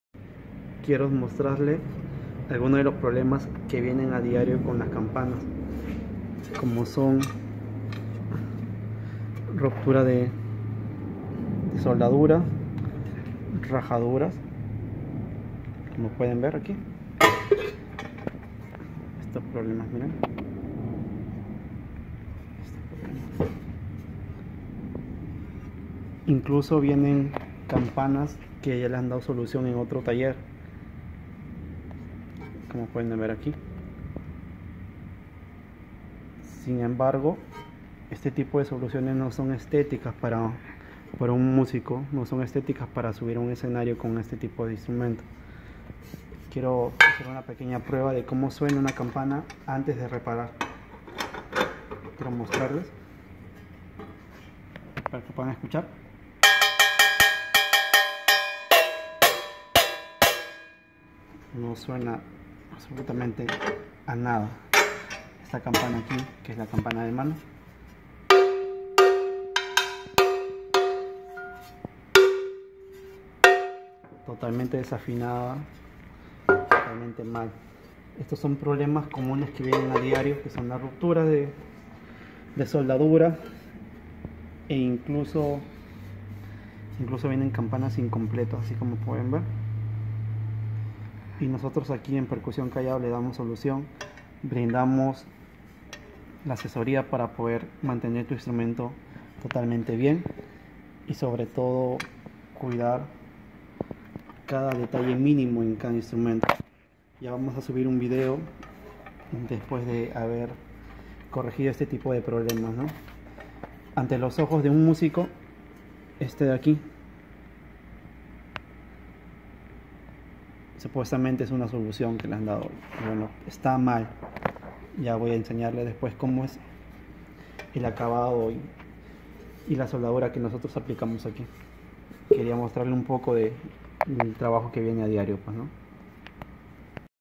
Campanas DAÑADAS VIDEO1
mostramos el proceso y las campanas que llegan al taller - CAMPANAS DAÑADAS